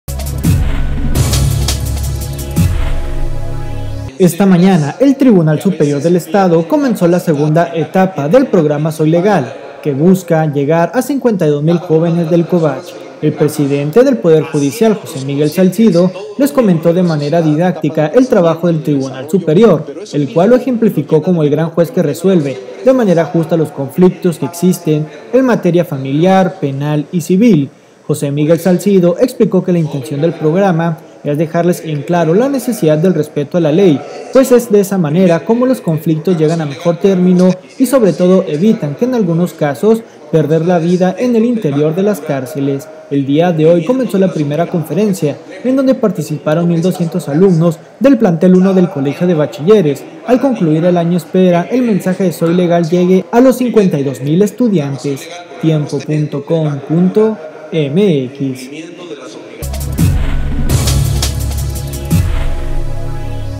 Tiempo TV | Llevan mensaje "Soy Legal" a 52 mil jóvenes del COBACH
Esta mañana el Tribunal Superior del Estado, comenzó la segunda etapa del programa Soy Legal, que busca ahora llegar a los 52 mil jóvenes del COBACH.